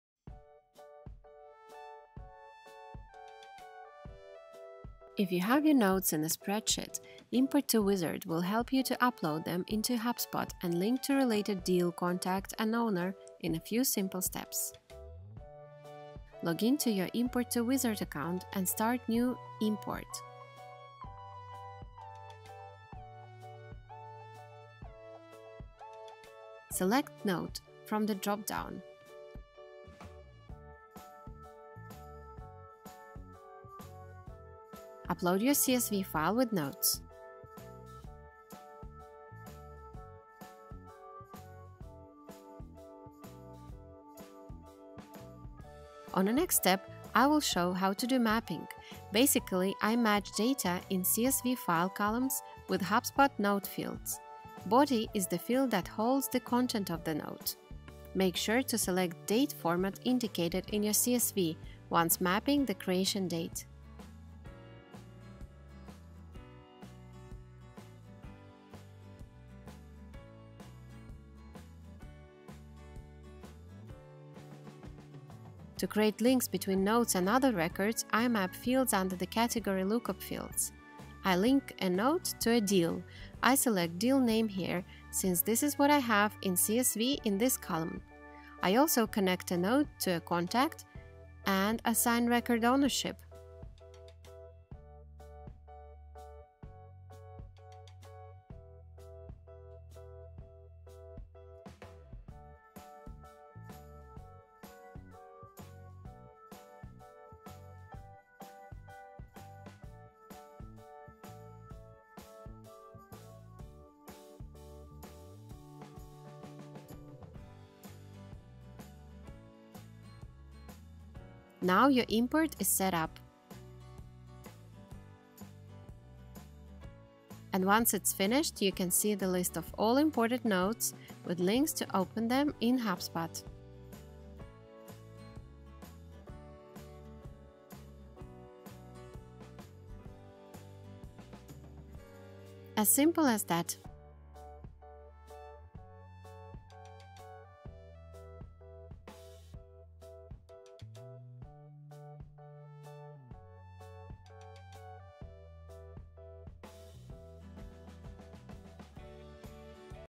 Import Notes into HubSpot (link to Contact and Deal)
With CSV Loader you can mass upload notes into HubSpot from a CSV file. No automation or triggers here. You can link Notes to Contacts, Companies, and Deals so they appear on the activity timeline. Follow simple steps and import business data where it's needed.